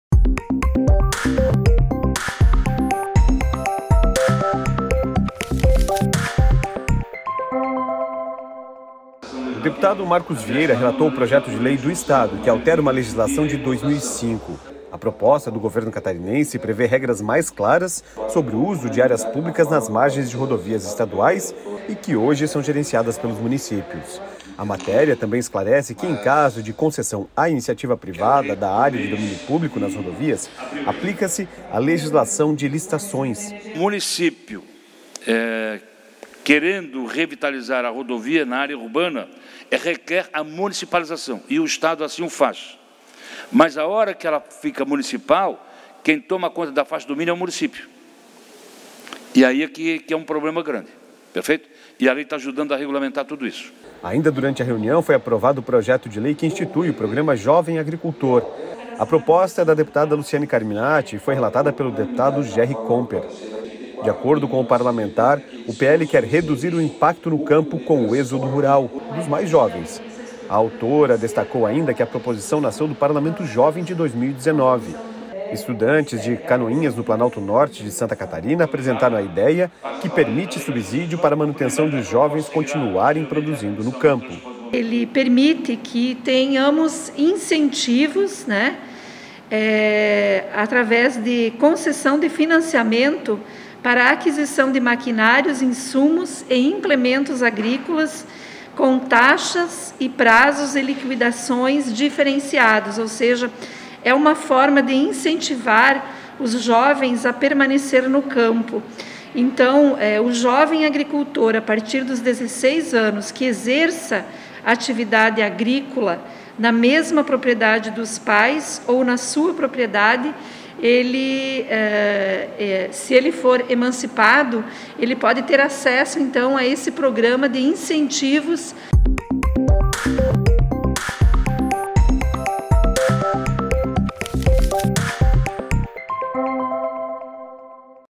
É Notícia - Projeto Jovem Agricultor é aprovado na Comissão de Finanças - 14/04/21
O Projeto que visa reduzir o êxodo rural foi aprovado na Comissão de Fianças da Assembleia Legislativa. A proposta vê ações de políticas públicas que incentivem jovens a partir de 16 anos, incluindo empréstimos facilitados para aquisição de maquinário.